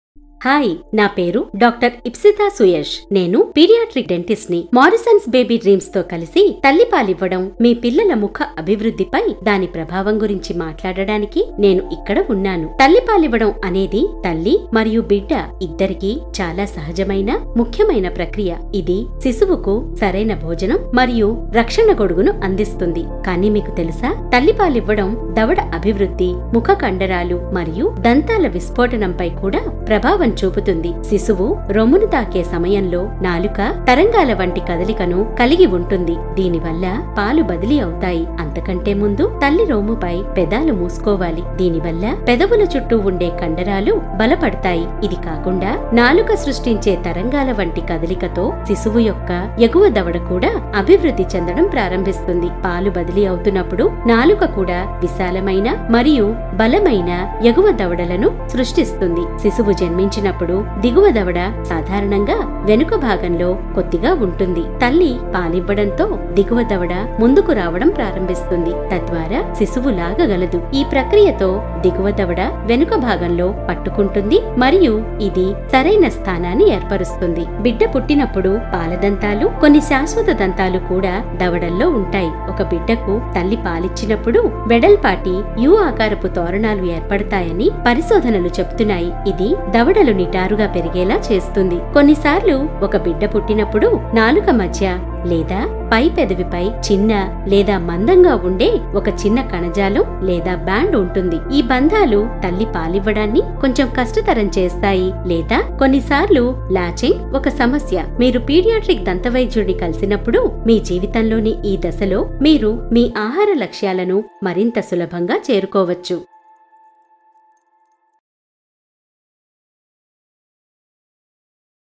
బిడ్డ facial muscles పై breastfeeding ప్రభావం | Dr. Ipshita Suyash | Morisons Baby Dreams | Telugu
ఈ వీడియోలో,paediatric dentist Dr. Ipshita Suyash శిశువులలో facial muscle, jaw development & teeth eruptionపై  breastfeeding ప్రభావం గురించి మాట్లాడతారు.
బిడ్డ తల్లి breastను నోటితో పట్టుకున్నప్పుడు, పెదవుల చుట్టూ ఉన్న 8 మజిల్స్ బలపడతాయి. breast milk లోపలికి వెళ్ళేటప్పుడు, టంగ్ యొక్క వేవ్ వంటి మోషన్ విశాలమైన మరియు ధృడమైన అప్పర్ జాస్ను సృష్టించేందుకు సహాయపడతాయి. శిశువు జన్మించినప్పుడు, లోవర్ జా సాధారణంగా కొద్దిగా వెనుకకు ఉంటుంది మరియు breastfeeding సహాయంతో, లోవర్ జా రైట్ పోసిషన్లో ముందుకు వస్తుంది.
కొన్నిసార్లు, టంగ్ మధ్య లేదా అప్పర్ లిప్పై స్మాల్ టిష్యూ ఉన్నందున, బిడ్డకు  breastfeed ఇవ్వడం కష్టం. ఈ టైస్ బిడ్డ నోటితో బ్రెస్ట్ను పట్టుకోవడాన్ని కష్టతరం చేస్తాయి. కానీ, మీరు  paediatric dentistని కలిసినప్పుడు, సరైన మార్గదర్శకత్వంతో, మీరు మీ ఫీడింగ్ గోల్స్ను మరింత సులభంగా చేరుకోవచ్చు.